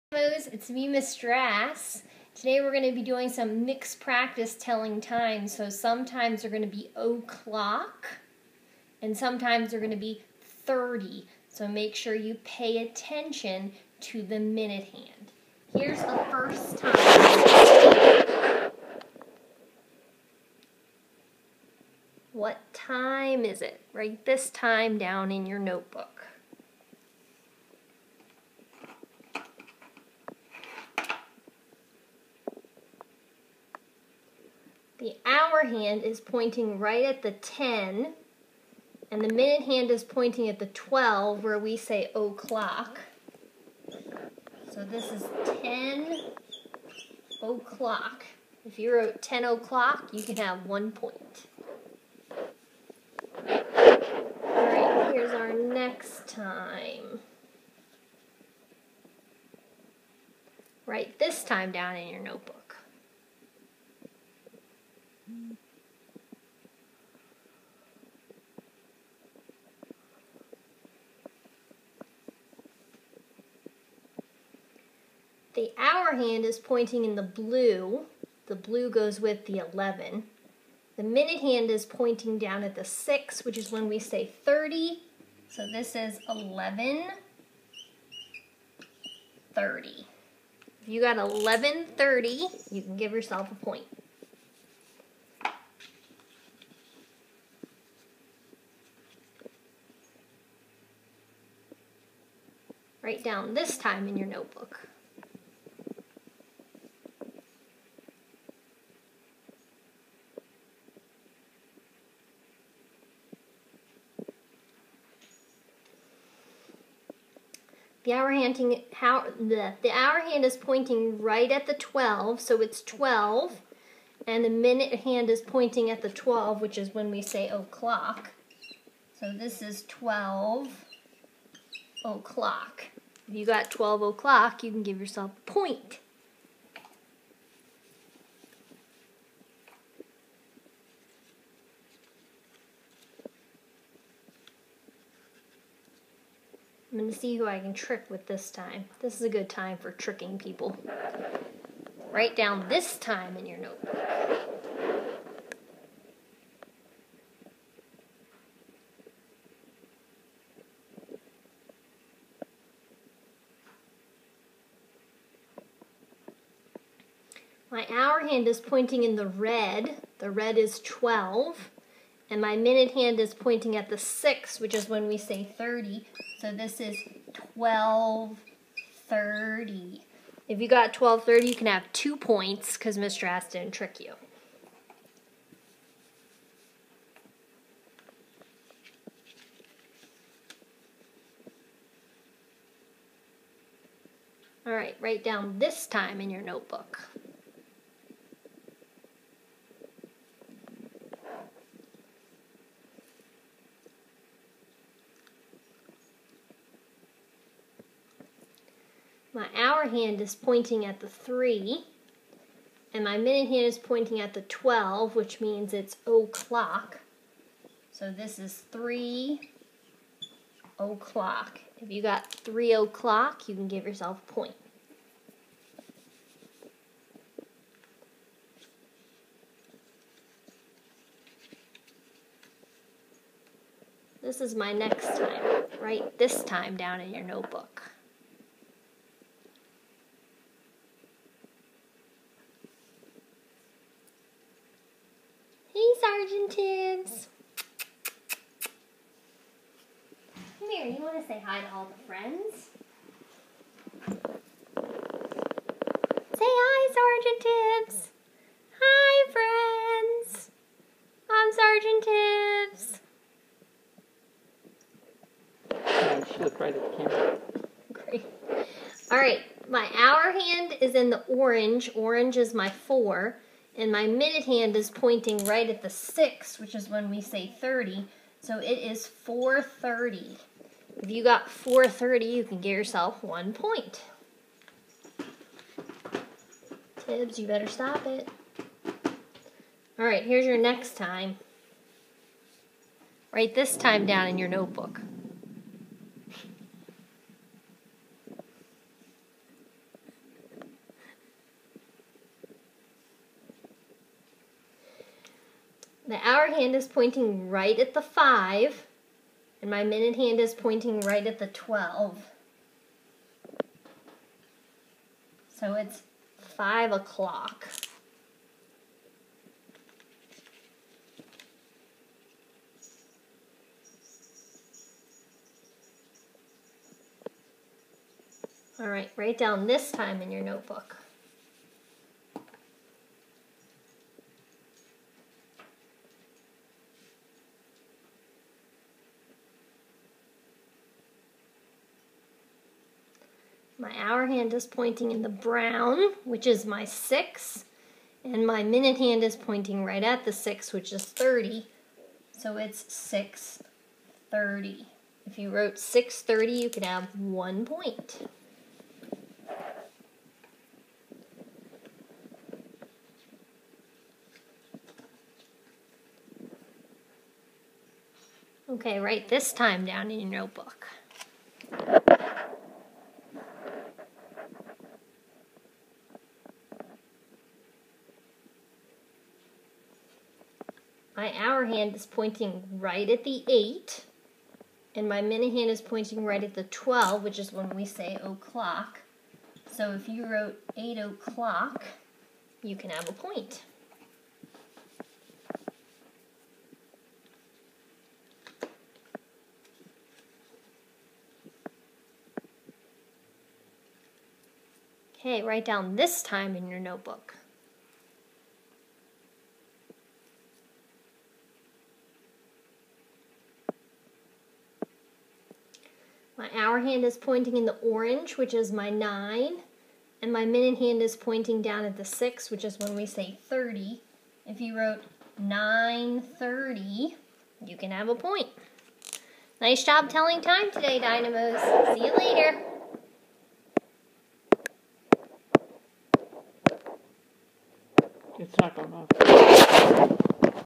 Telling Time 5/7
Practice telling time to the hour and half hour with Miss Drass.